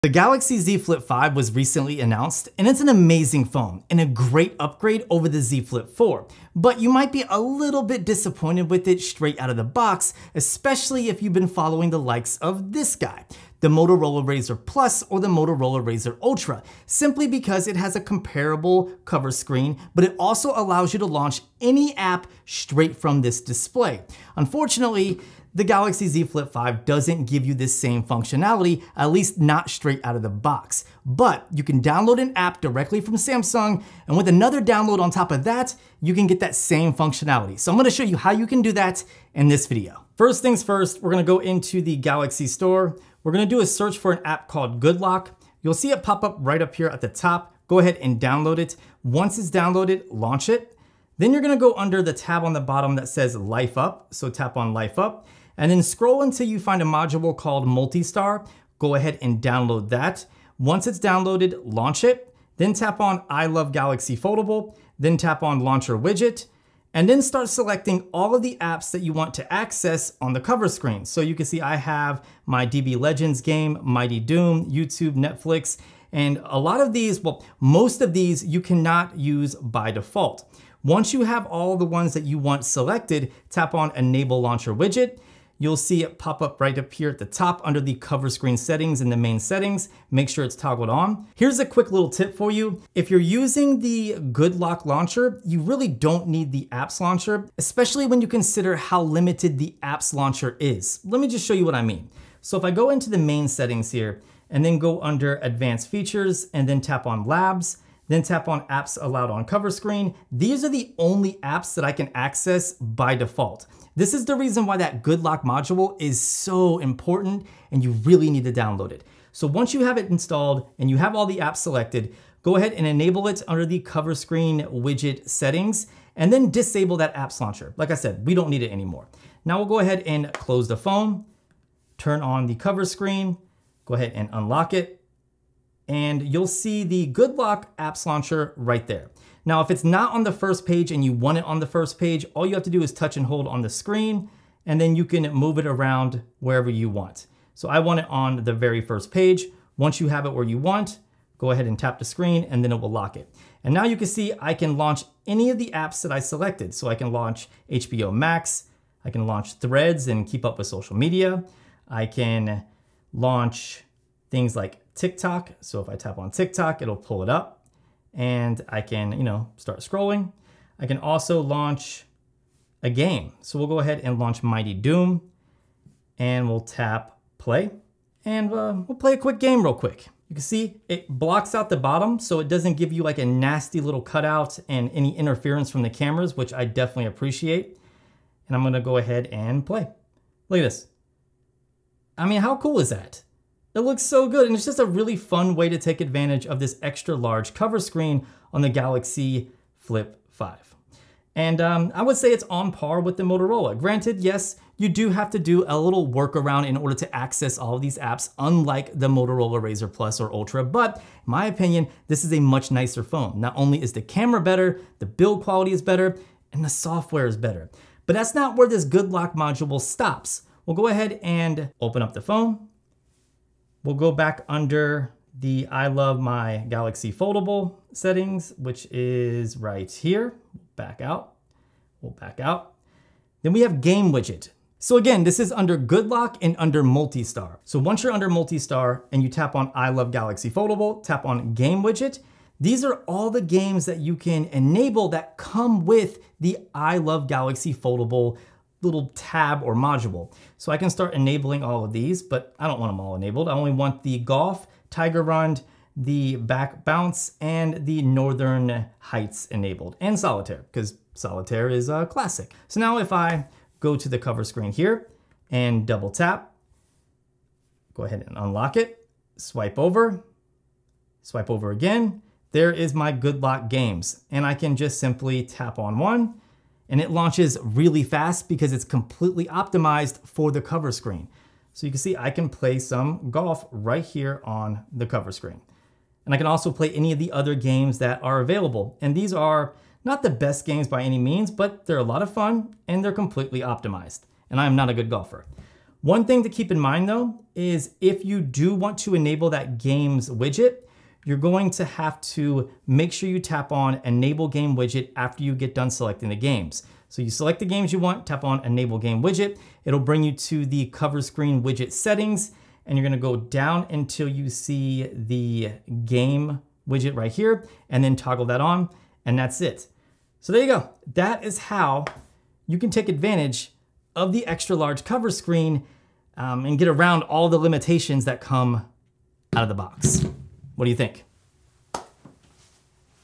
Galaxy Z Flip 5 - How To Use Any App On The Cover Screen!
Did you know that you can actually launch any app from the cover screen of your Galaxy Z Flip 5? It's true! Just download two apps from the Galaxy Store: Good Lock and MultiStar. Once you have those installed, you can select the apps you want to be able to launch from the cover screen. You can also add a game widget to the cover screen, which gives you quick access to a selection of games that are optimized for the small screen.

To do this, just follow these steps:

Download the Good Lock and MultiStar apps from the Galaxy Store.
Open the MultiStar app and select the "I Love Galaxy Foldable" setting.
Tap on "Launcher Widget"
Select the apps that you want to be able to launch from the cover screen.
Enable the "Game Widget" module in the MultiStar module under "I Love Galaxy Foldable"
Swipe to the right on the cover screen to see the list of apps that you have enabled.
That's it! Now you can enjoy all of your favorite apps on the cover screen of your Galaxy Z Flip 5.

I hope this helps! Let me know if you have any other questions.

Galaxy S22 Ultra vs Z Flip 4 vs Z Fold 4 - Which Smartphone Should You Buy?